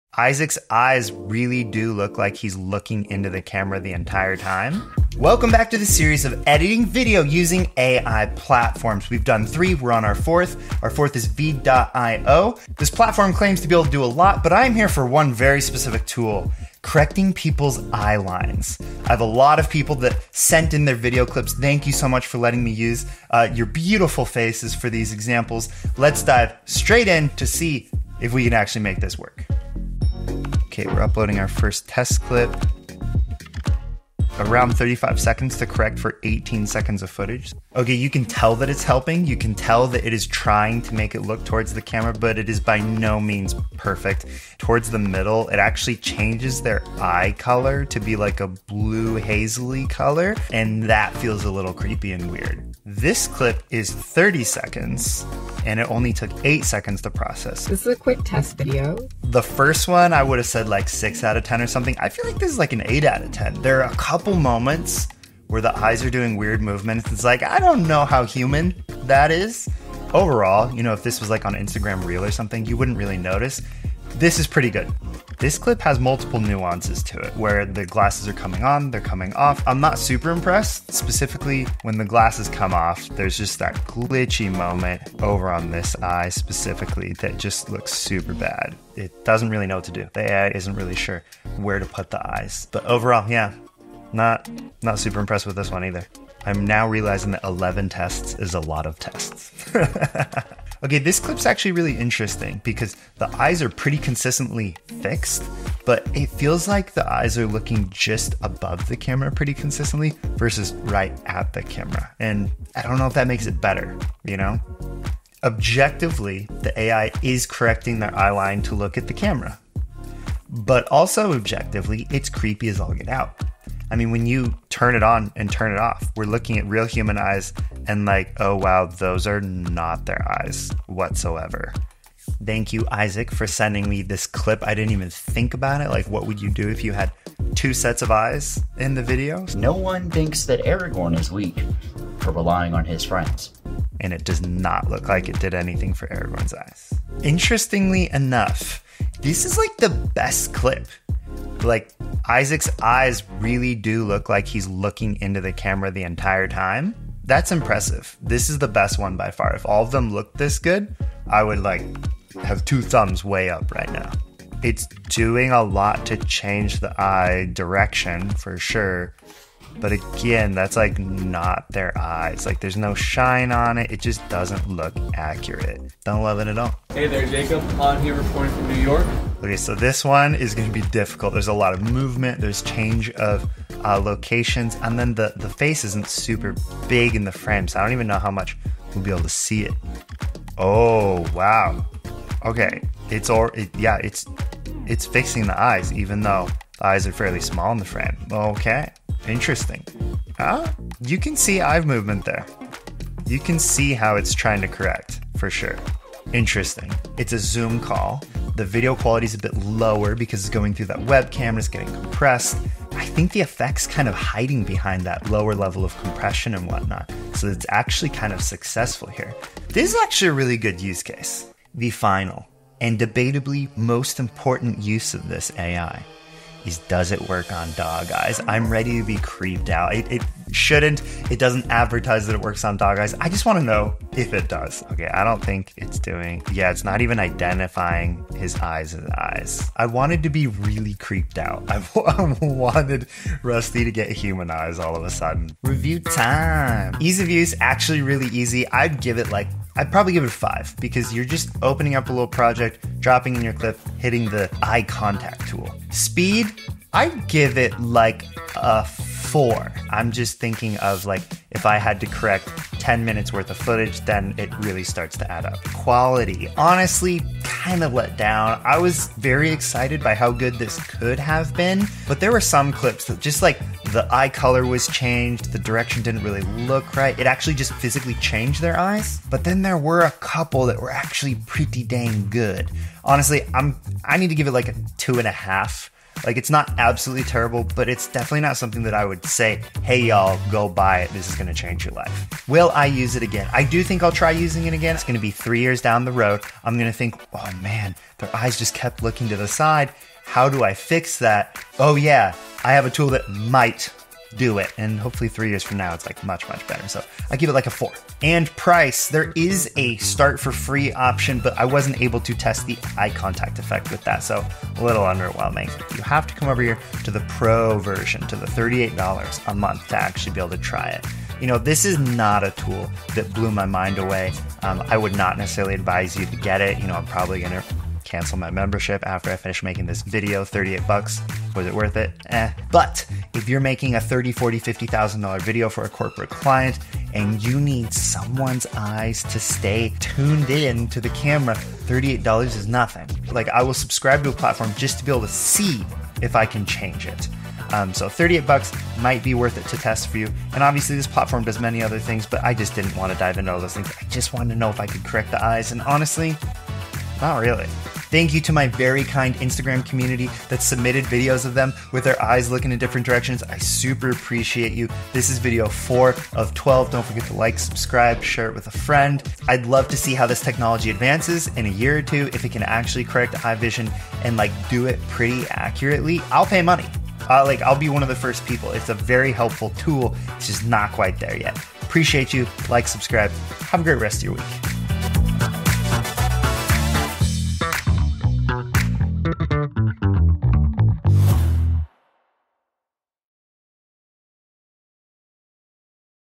EYE CONTACT AI TOOL - does it actually work?
0:00 - Overview
0:29 - The First Test
0:57 - Test 2
1:54 - Test 6
2:11 - Two Face Test
2:44 - Test 8
2:56 - Walking And Talking Test
3:41 - Dog Eye Test
4:05 - OFFICIAL Ratings
5:57 - Wrap Up

18x award-winning Creative Director, Jacob K Christensen, reviews VEED.IO, an AI-powered eye correcting tool.